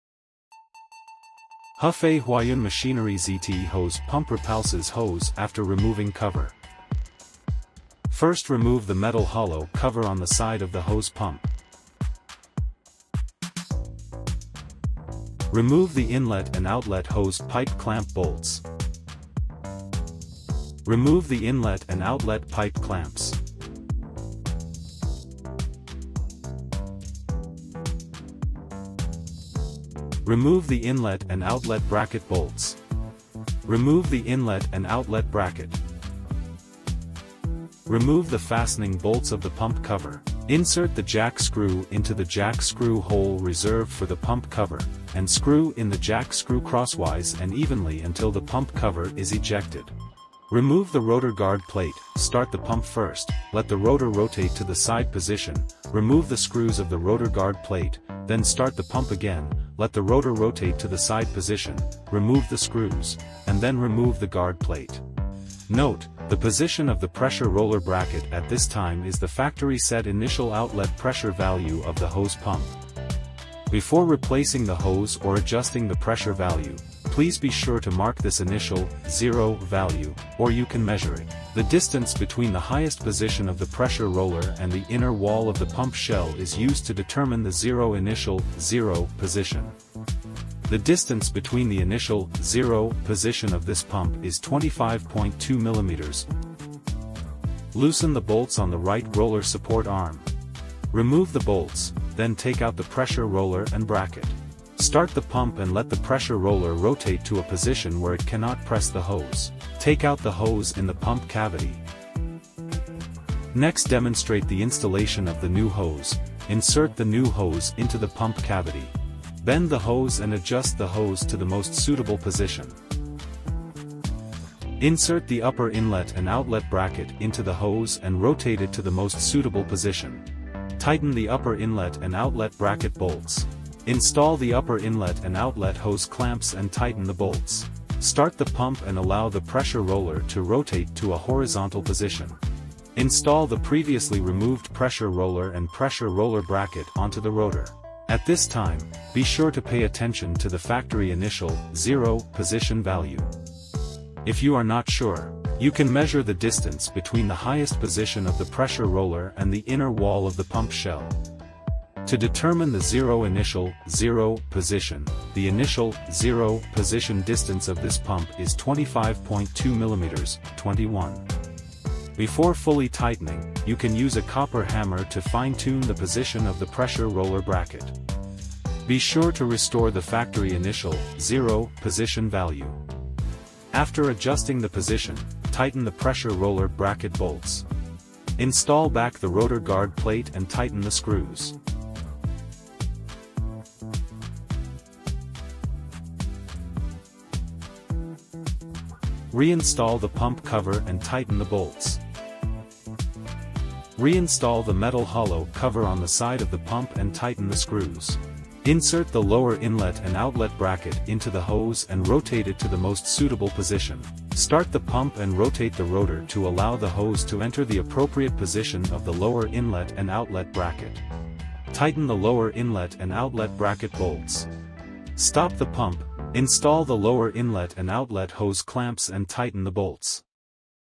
How to replace the hose of a peristaltic hose pump|Huayun ZT Industrial peristaltic hose pump
How to replace the hose of a peristaltic hose pump is a key daily maintenance of the peristaltic hose pump. This video will show the detailed hose replacement method and steps of Huayun peristaltic hose pump.
If you want to find videos related to Huayun brand and peristaltic hose pump.

timeline:
00:00 Preparation before replacing the hose
01:09 Precautions when replacing hoses
01:33 Remove the old hose
01:54 Install new hose
02:30 Pressure reset adjustment
03:15 Parts reset and installation